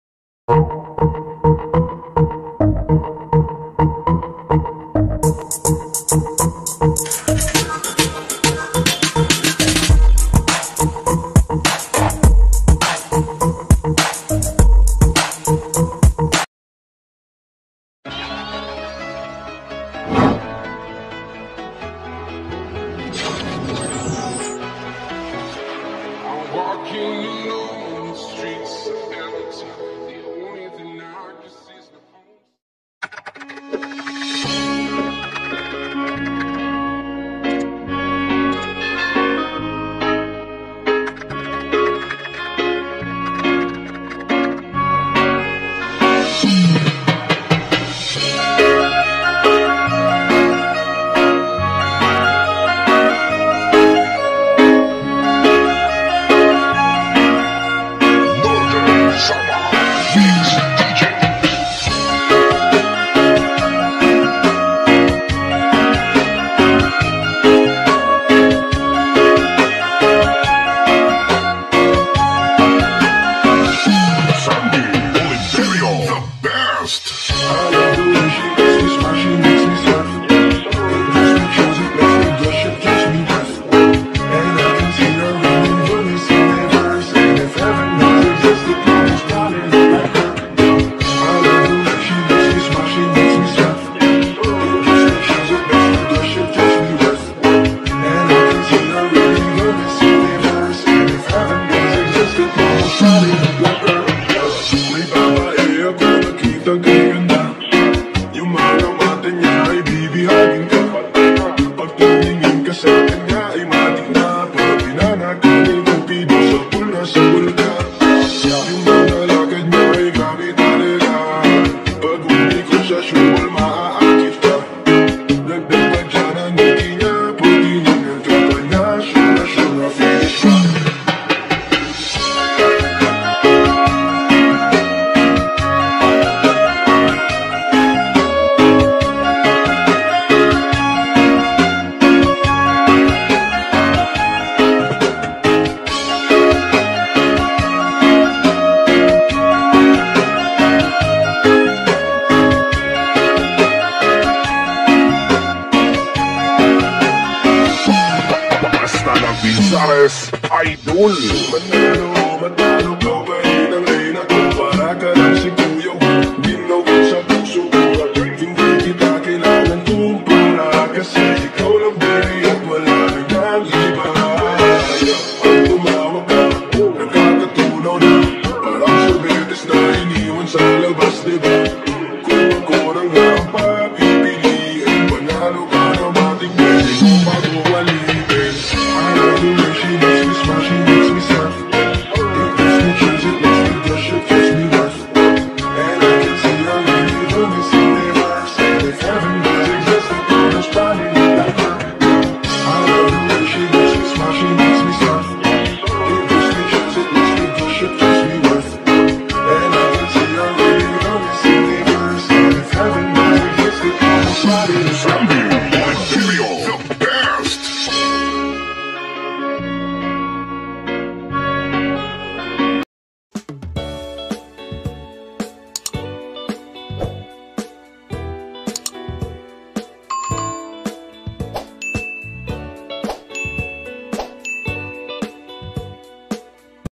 Going to Job Site
nalibot Kona ang mga magagandang lugar dto sa hail sa kakabeyahi ko araw araw papunta sa duty ng ibat ibang kabahayan dahil sa akong trabaho